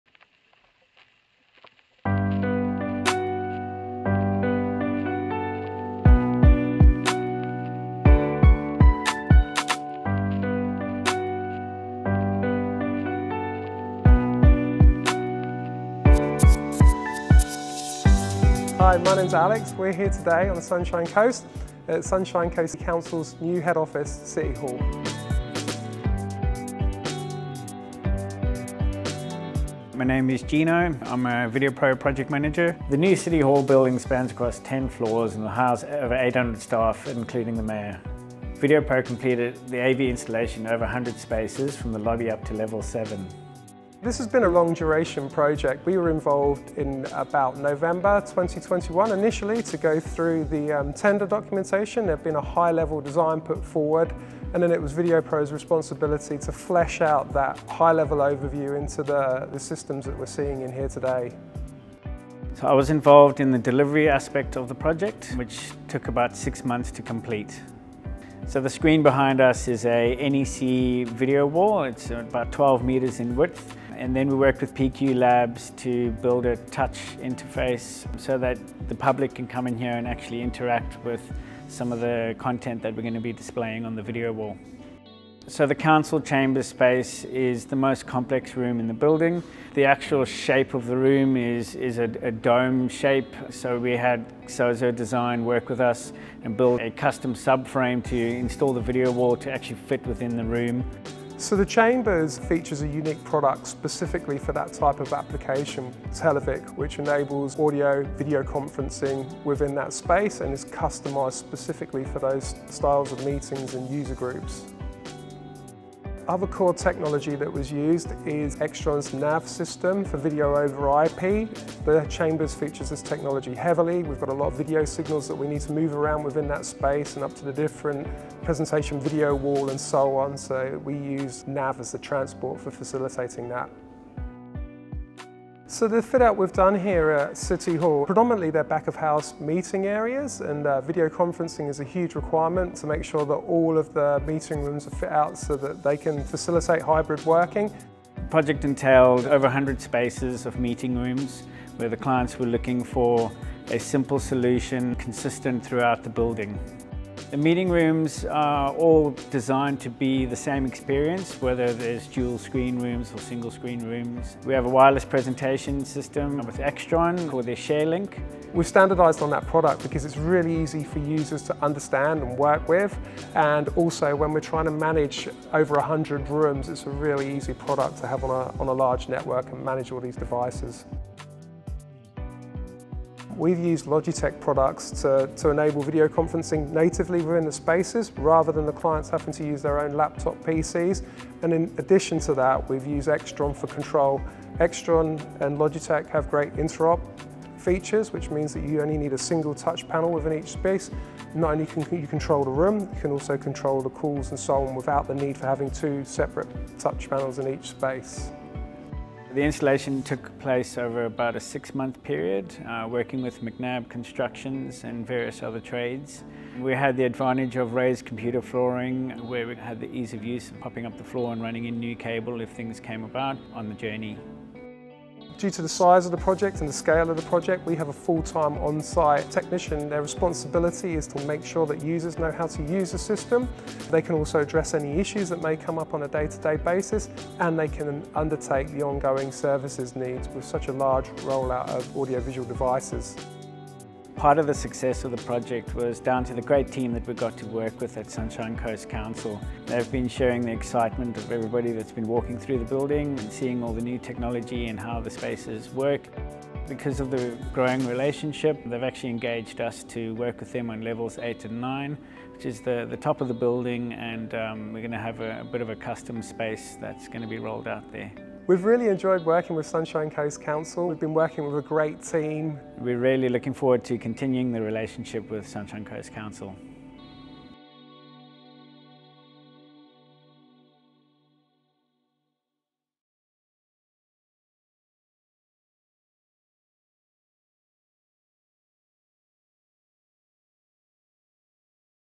Sunshine Coast Council City Hall | Audio Visual Case Study
The new Sunshine Coast Council City Hall building spans across ten floors and houses over 800 staff, including the mayor. Videopro completed the AV installation in over 100 spaces from the lobby up to level seven.

The lobby at City Hall features an impressive interactive NEC video wall, spanning 12 meters in width. The video wall can be used by the Council to display custom content and provides a huge wow-factor when entering the building. Videopro worked with PQ Labs to build a touch interface so the public can interact with the content displayed on the video wall.
The  Council  Chambers  is  the  most  complicated  space  in  the  building. A large, curved NEC video wall is featured at the front of the room. Sozo Design worked with Videopro to build a custom subframe to accommodate the  unique  dome  shape  of  the  room  itself.  The  Chambers  features  a unique product specifically designed for this type of application. Televic tabletop  units  enable  audio  and  video  conferencing  within  the  space. Each Council member has their own 10” tiltable touch screen interface and microphone, enabling the user to have simple control of this complex space. The other core technology used in the Council Chambers is the Extron Nav system for video over IP. The Chambers features this heavily as there are a large number of video signals that need to move around within the space and to the different display options.

The City Hall project consists predominantly of back of house meeting areas.  Video  conferencing  is  a  huge  requirement  to  facilitate  Sunshine Coast  Council’s  hybrid  working  model.  There  are  over  100  meeting room spaces, so the client was looking for a simple, consistent solution throughout the building. All the meeting rooms are designed to be the same experience whether a dual or single screen room, huddle space or large combinable room.

The  meeting  room  wireless  presentation  system  is  standardised  with Extron ShareLink Pro. The product is very easy for users to understand and  work  with  and  is  a  straightforward  product  to  manage  on  a  large network. Logitech products have been used to enable video conferencing natively within the spaces, rather than the clients having to use their own laptops. Room control is then managed by Extron products. Extron and Logitech have great interoperability features, meaning you can control not only the room, but also the VC call with only a single touch panel within each space.

Part  of  the  success  of  the  project  was  down  to  the  great  team  that Videopro  worked  with  at  Sunshine  Coast  Council.  Due  to  the  growing relationship, they have engaged Videopro to complete the audio visual solutions on levels eight and nine.

Looking for help designing or upgrading your corporate or education space?
or 1300 VIDEOPRO. For more information on our services and solutions,